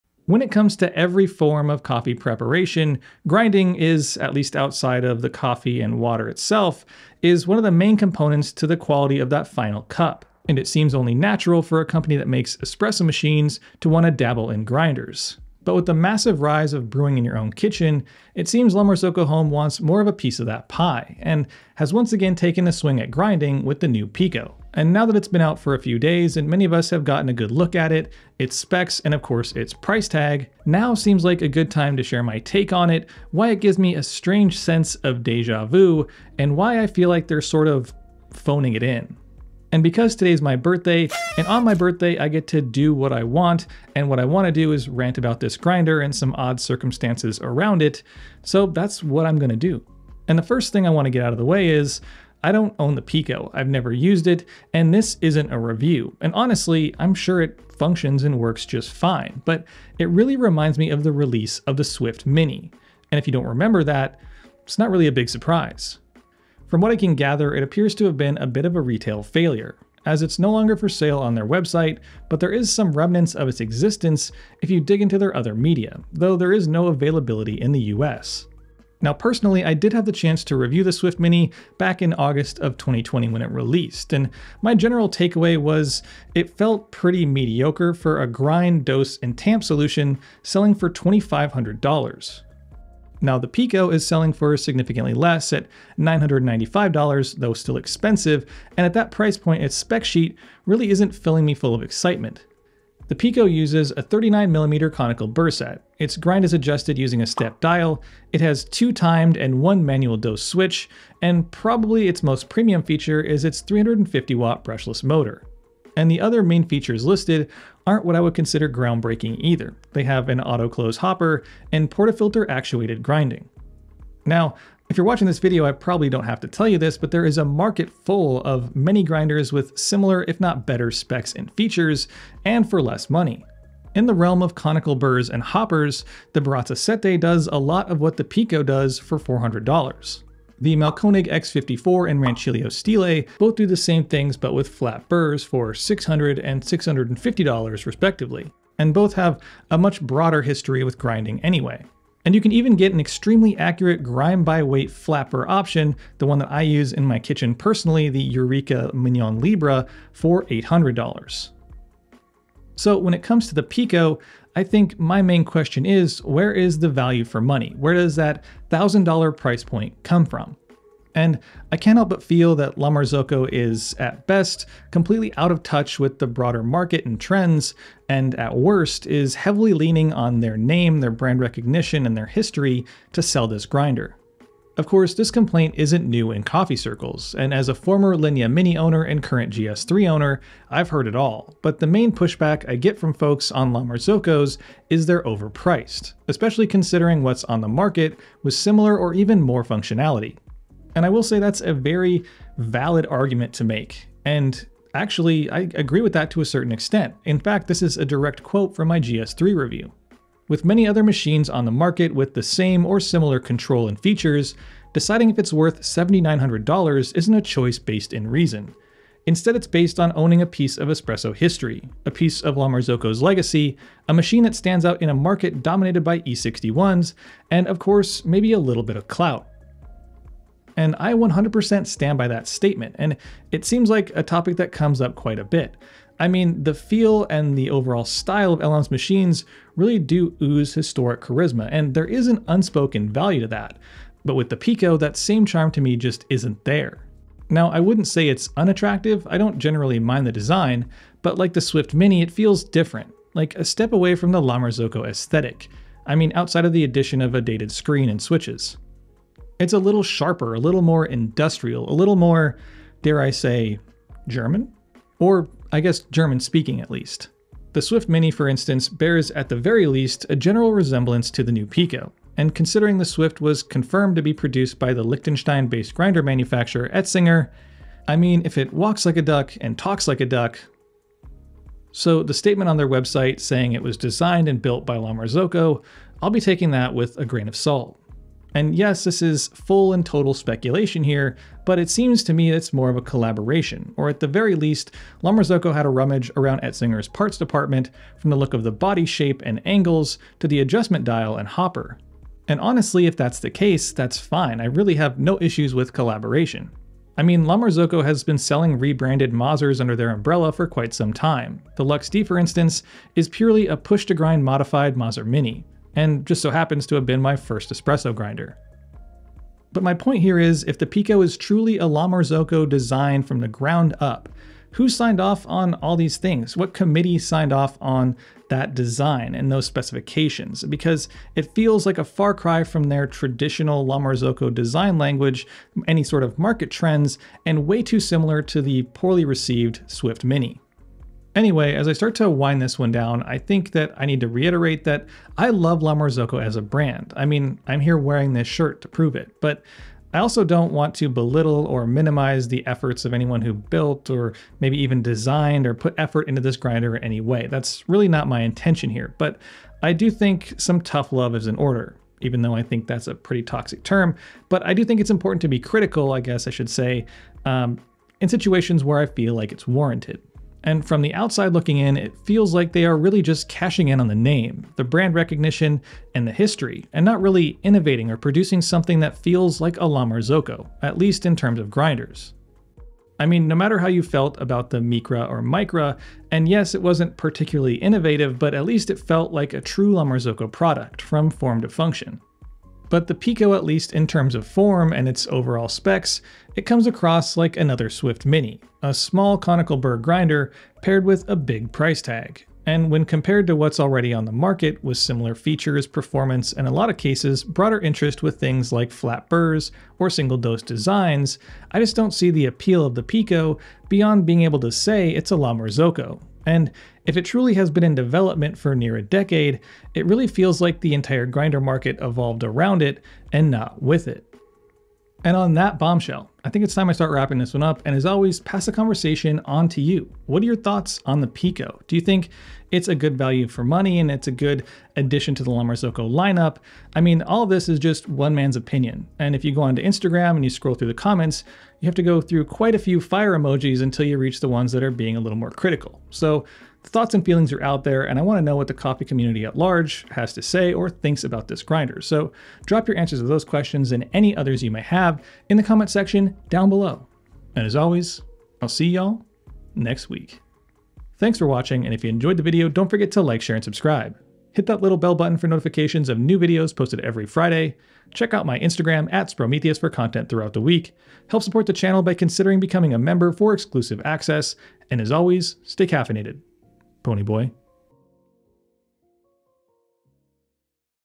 La Marzocco Pico | How Did This Grinder Take 10 Years To Develop?
La Marzocco has always played a major role in my coffee experience, from a home barista, to a professional...something...and they've also held a special place in my heart. I love the brand, but some decisions they make can really leave me scratching my head and underwhelmed; and now the Pico espresso grinder has entered the chat. Hold on to your hats, because I've got thoughts.

Help me create content and pay bills.